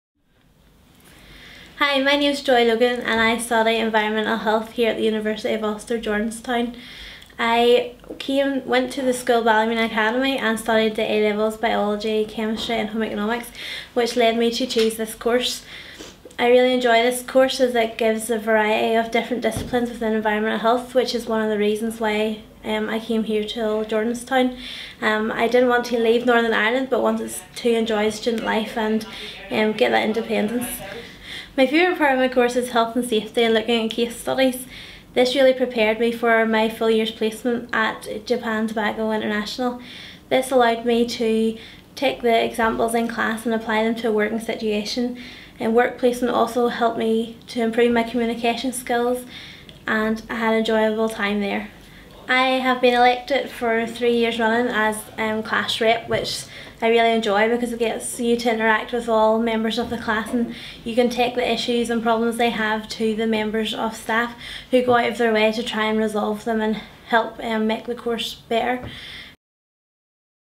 Student Profile : Joy Logan - BSc Hons Environmental Health
A short interview with environmental health student Joy Logan. Joy talks about her time on the course an on her placement at Japan Tobacco.